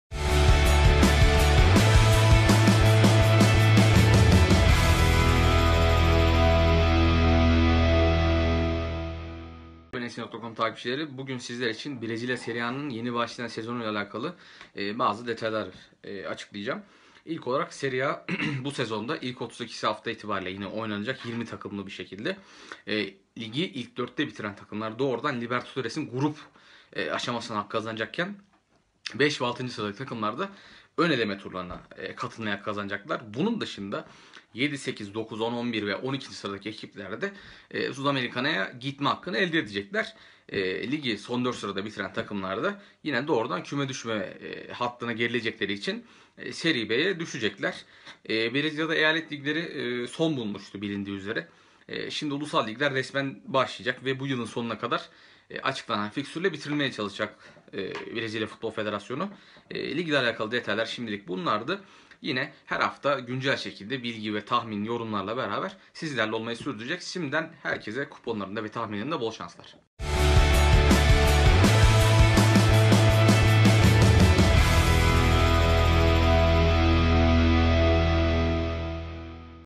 Mertcan Bulca | Brezilya Serie A Bilgileri
Mertcan Bulca, yeni başlayacak olan Brezilya Serie A'yı sizler için yorumladı.